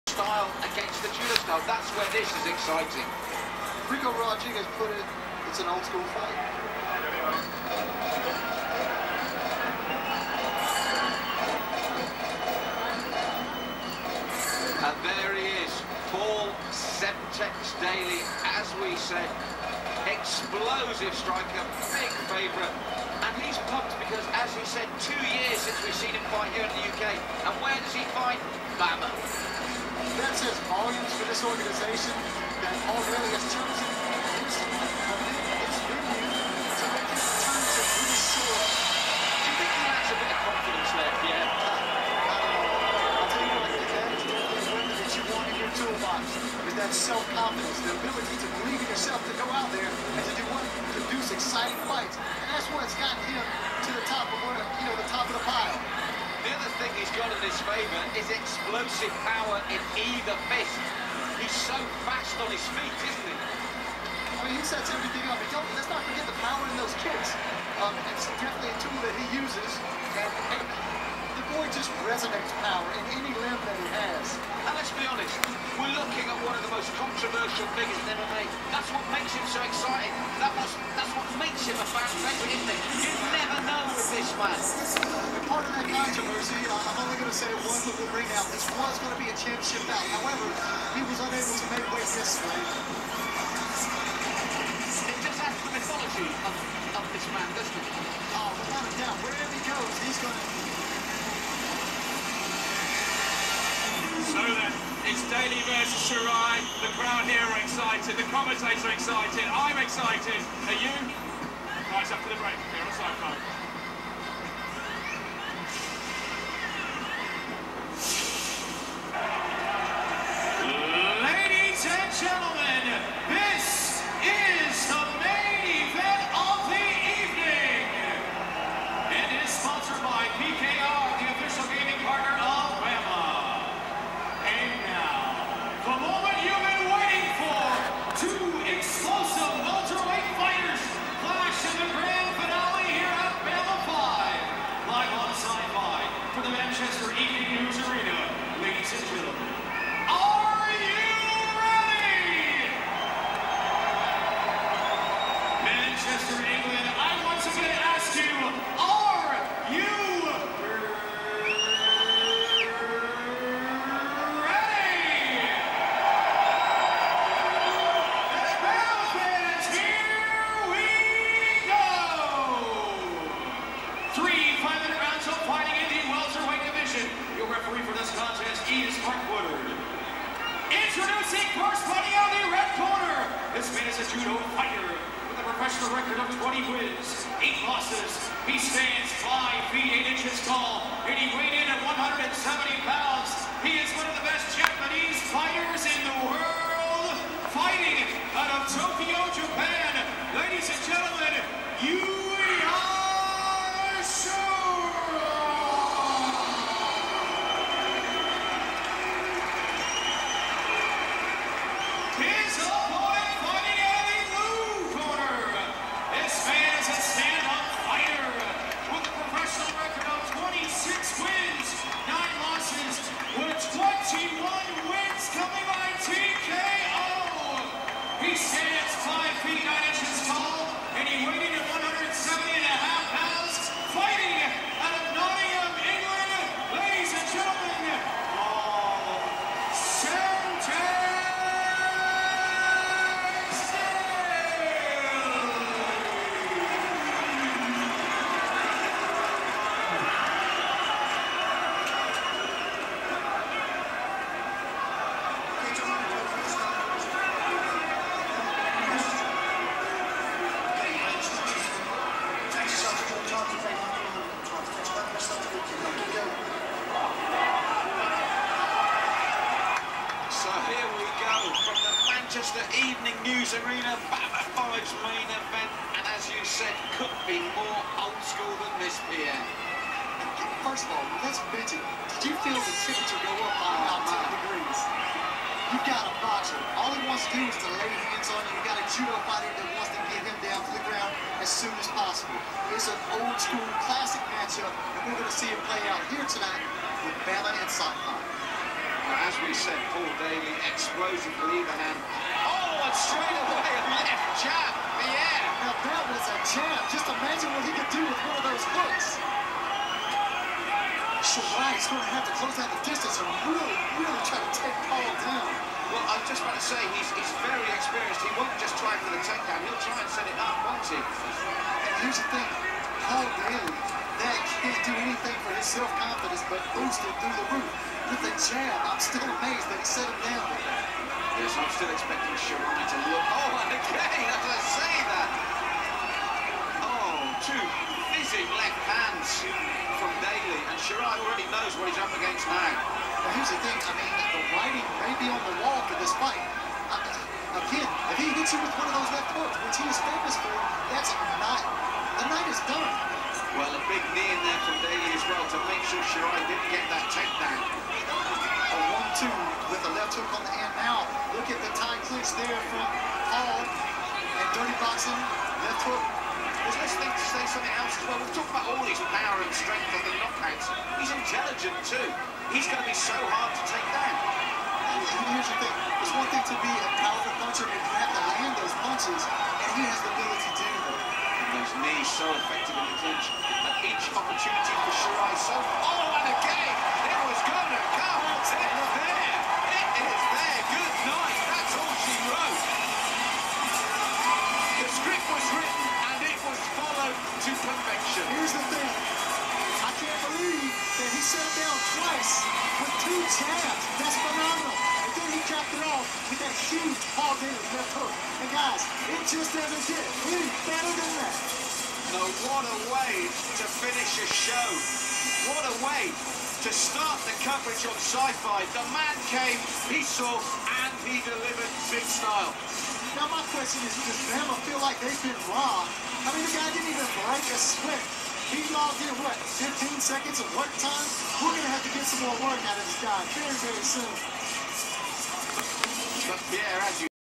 Paul Daley vs. Yuya Shirai
Paul Daley fights Yuya Shirai in BAMMA